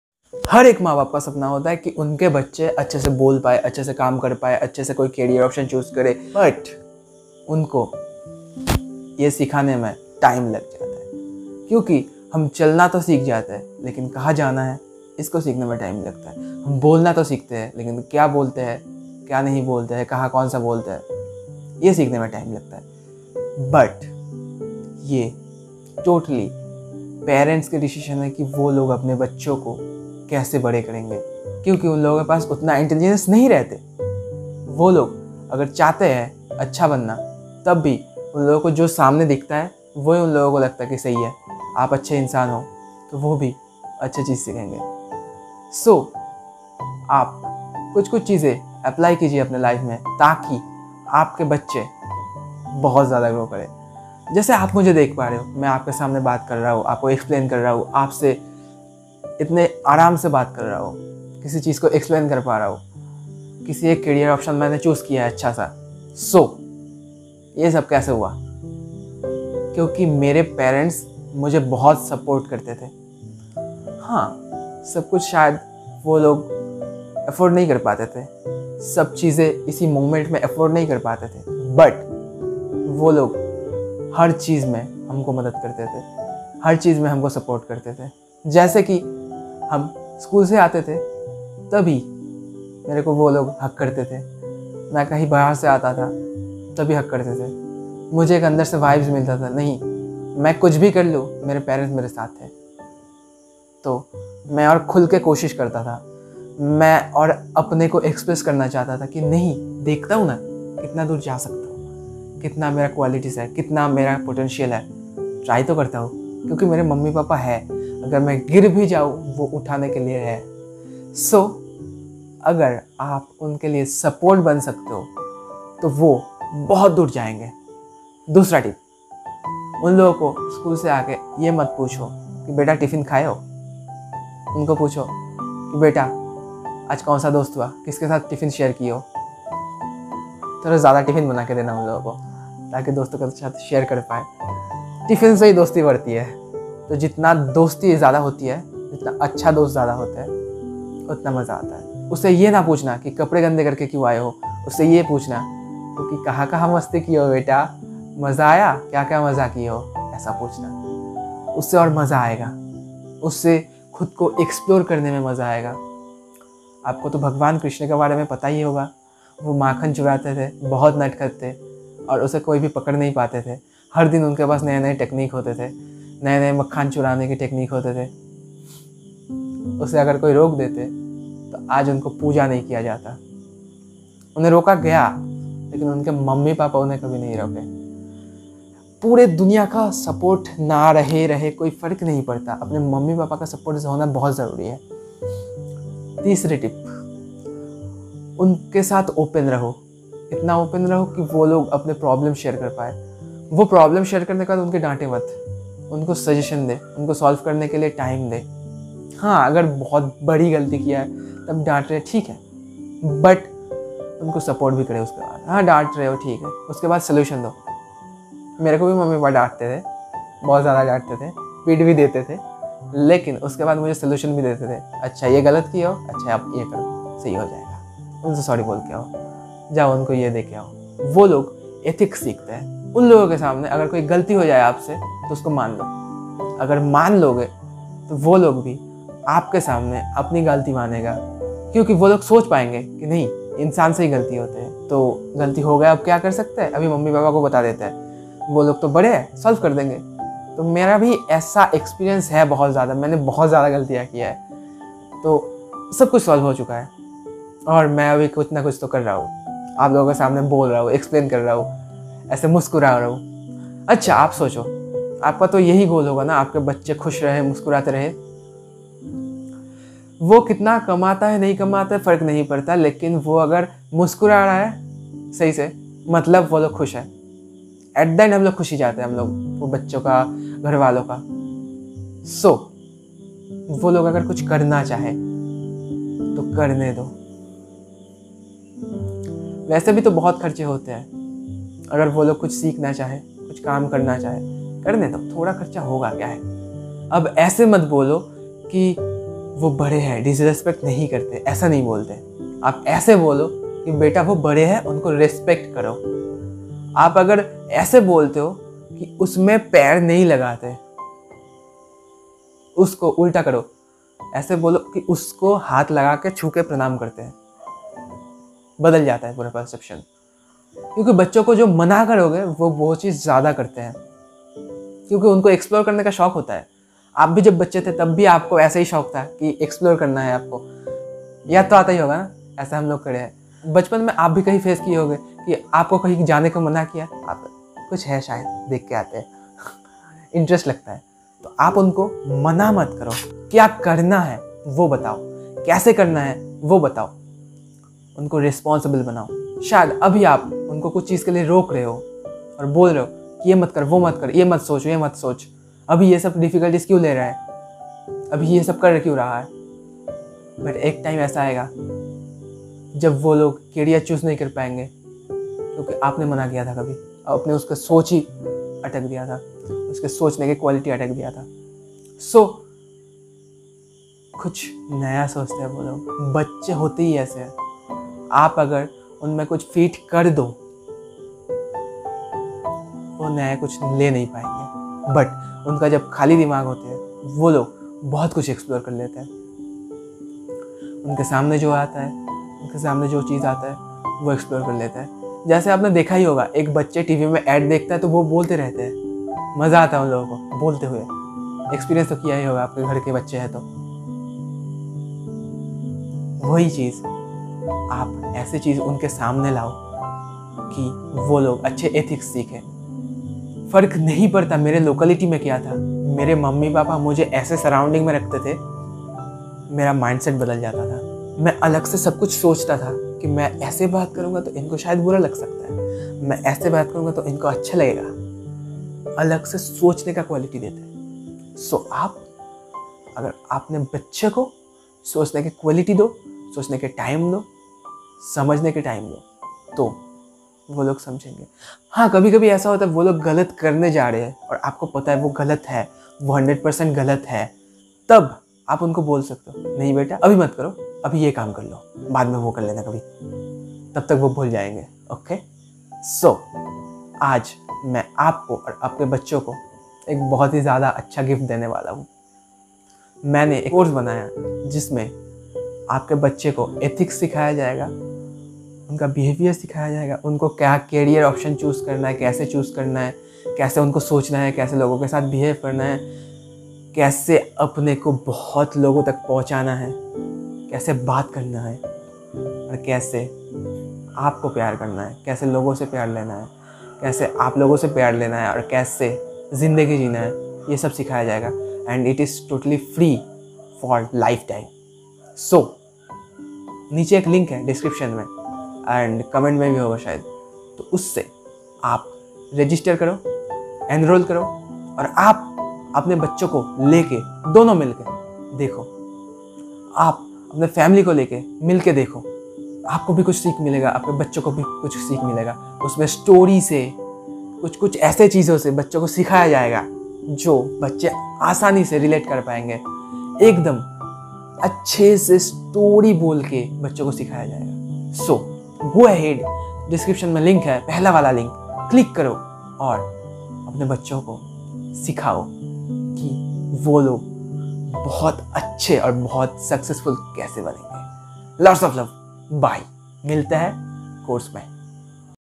Step-by-Step PROCESS to make Your Child RESPONSIBLE (in Hindi) With a GIVEAWAY by Subhojit Guha Roy
अपने बच्चे को जिम्मेदार बनाने के लिए कदम दर कदम प्रक्रिया (हिंदी में)

FREE FOR KIDS
Brings Success In Life

अपने सपनों को साकार करें
बच्चों के लिए मुफ़्त
जीवन में सफलता लाता है

जाओ और अभी नामांकन करो यह पूरी तरह से मुफ़्त है ...

अपने बच्चे को जिम्मेदार बनाने के लिए कदम दर कदम प्रक्रिया (हिंदी में) / एक उपहार के साथ